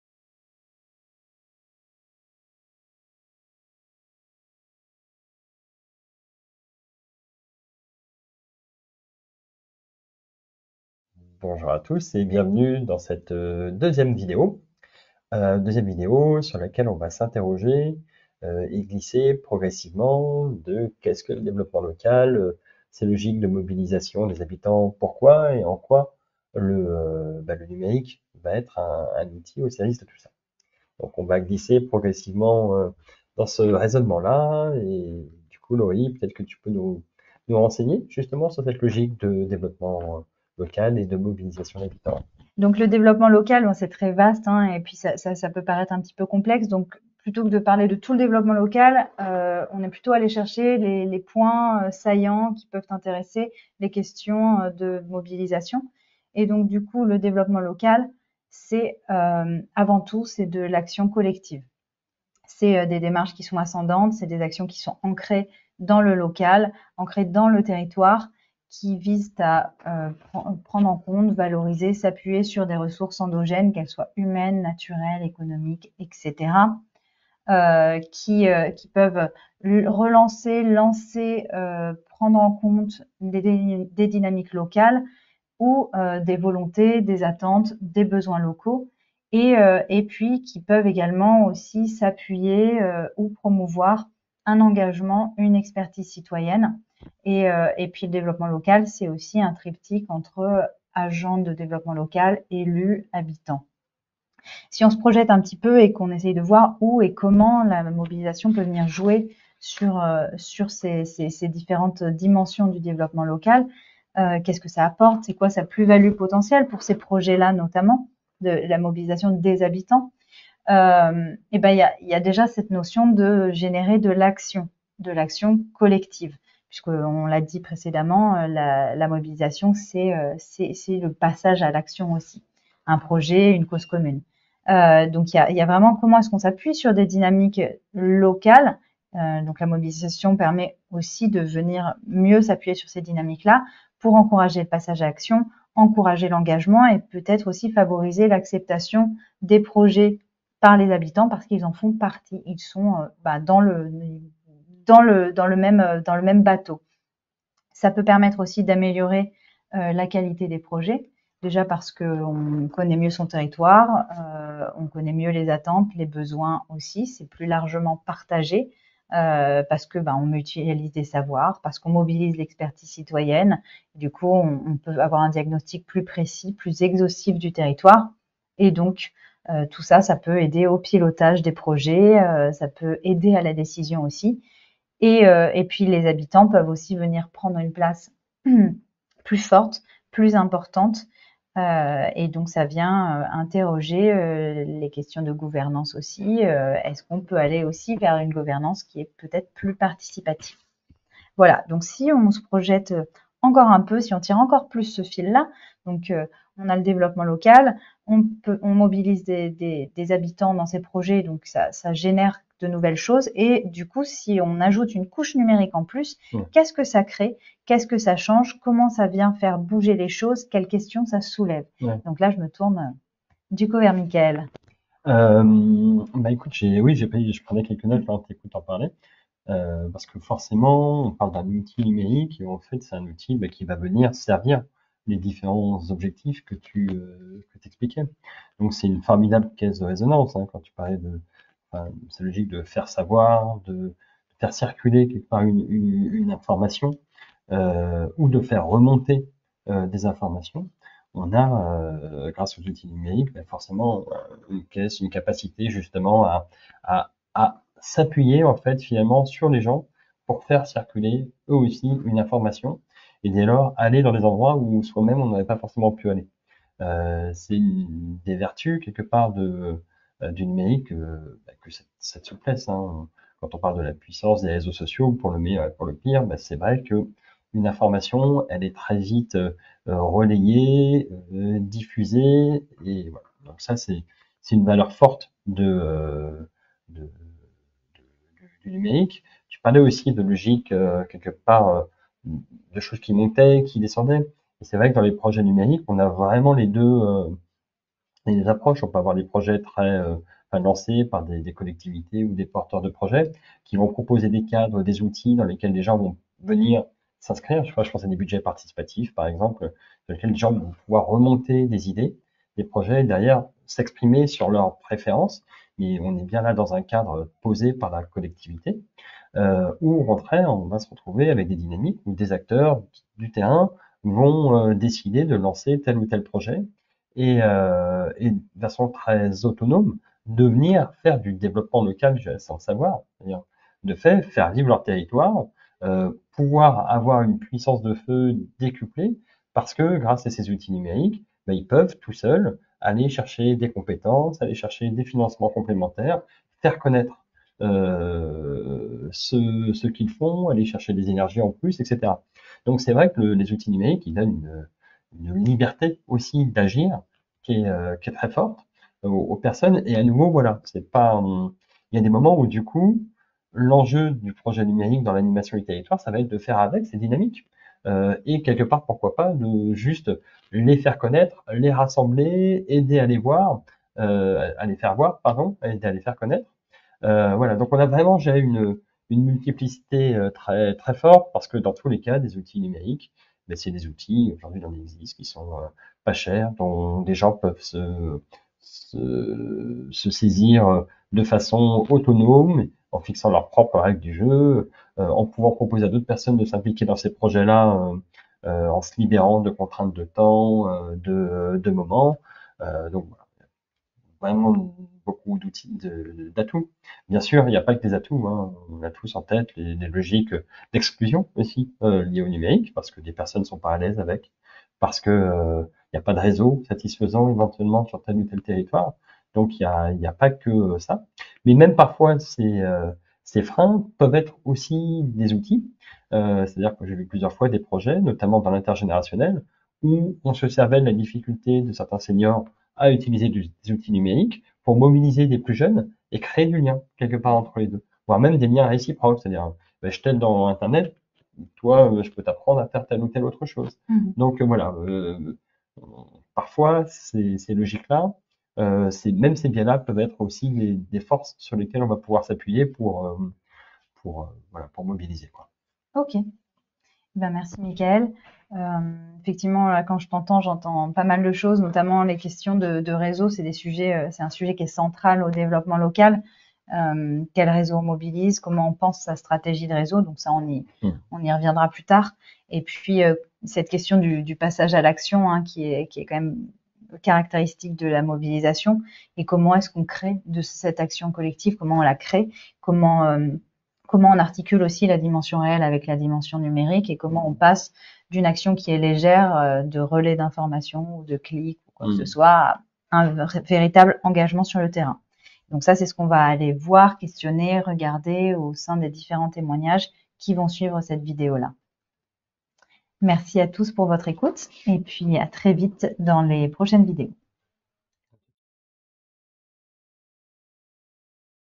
Mailler développement local, mobilisation des habitants et numérique
Dans cette vidéo - de la session 2023 de Cap Rural « Mobiliser les habitants dans des projets de développement local par le numérique » -, les questions suivantes sont abordées : pourquoi mobiliser les habitants dans des projets de développement local ? Pourquoi utiliser le numérique pour mobiliser les habitants ? Qu’est-ce que cela vient changer ? Quelles questions cela soulève ?